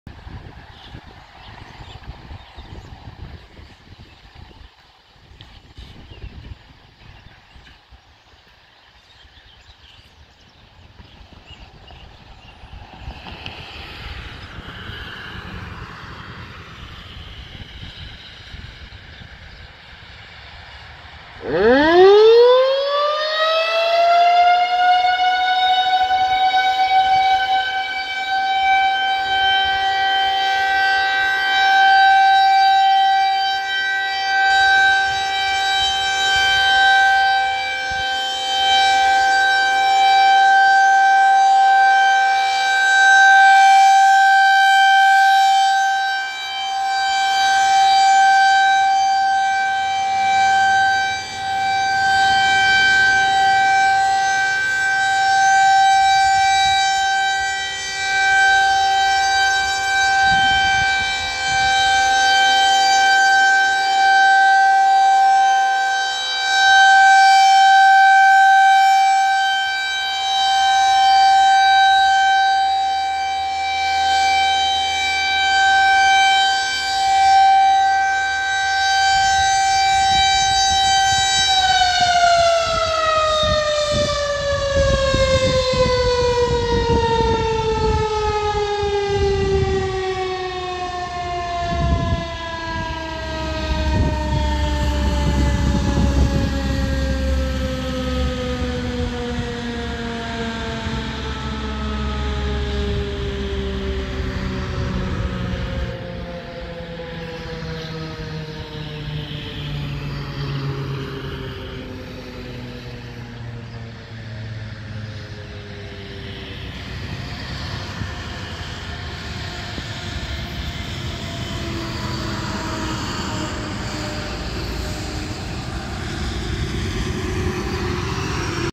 Federal Signal 2001-SRNB Alert-30 Corydon Kentucky at Henderson County Kentucky
this is the first time that no one recorded at Corydon Kentucky this is the 2001 SRNB it was originally place in fire department but then they move it to the water Tower. subscribe for more siren videos click like hit that notification button and Commet down below

©SoundLiker13 2024 Reuse allowed with permission only